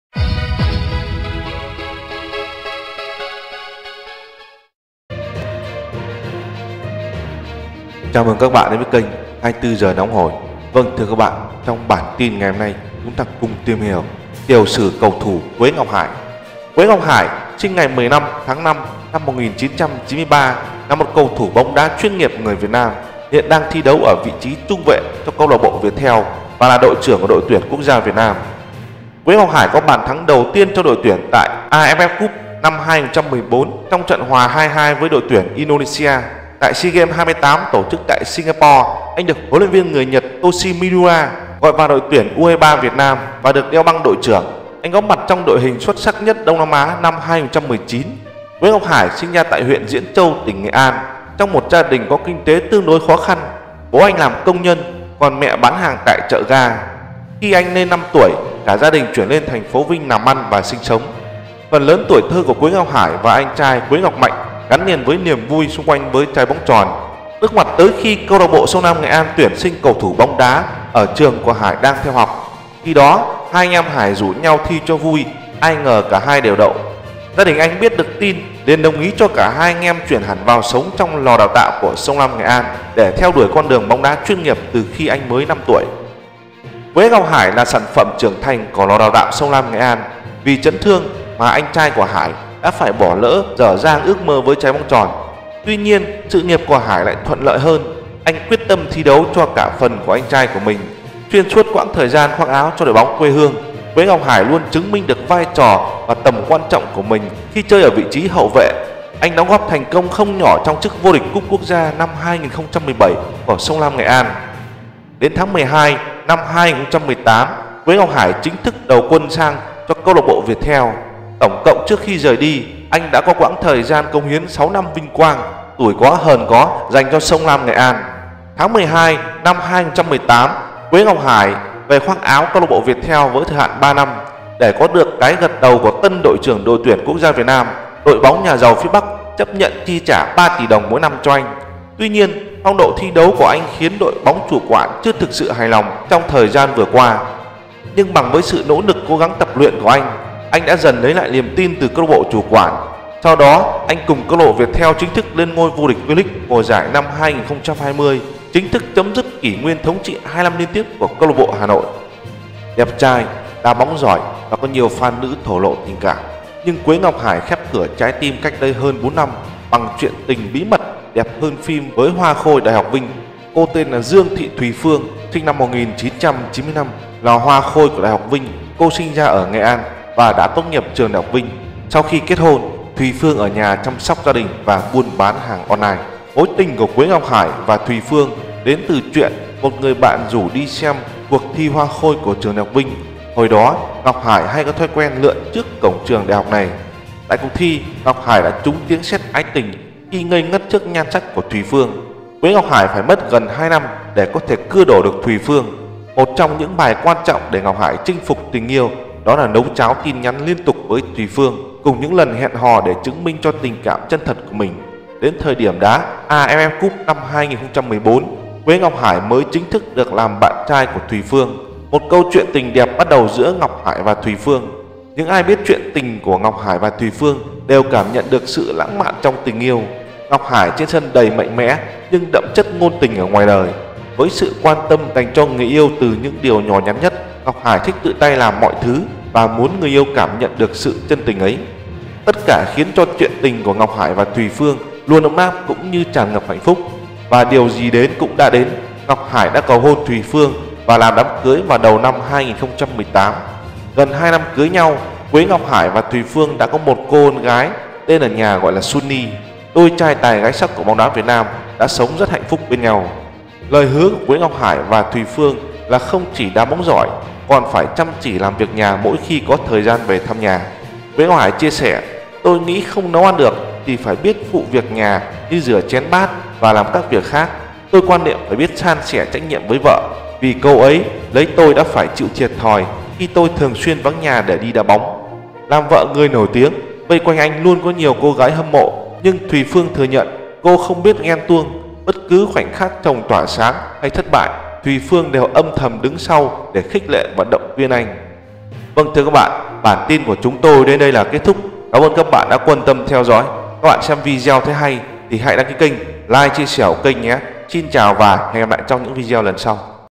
Tiểu Sử Cầu Thủ Quế Ngọc Hải Sau Cú Vấp Ngã – Tự Tin – Bản Lĩnh Đứng Dậy
Quế Ngọc Hải (sinh ngày 15 tháng 5 năm 1993) là một cầu thủ bóng đá chuyên nghiệp người Việt Nam hiện đang thi đấu ở vị trí trung vệ cho câu lạc bộ Viettel và là đội trưởng đội tuyển quốc gia Việt Nam.
Quế Ngọc Hải có bàn thắng đầu tiên cho đội tuyển tại AFF Suzuki Cup 2014 trong trận hòa 2-2 với Indonesia.[2] Tại SEA Games 28 tổ chức tại Singapore anh được huấn luyện viên Toshiya Miura gọi vào đội tuyển U23 Việt Nam tham dự và được đeo băng đội trưởng. Anh góp mặt trong đội hình xuất sắc nhất Đông Nam Á 2019.[3]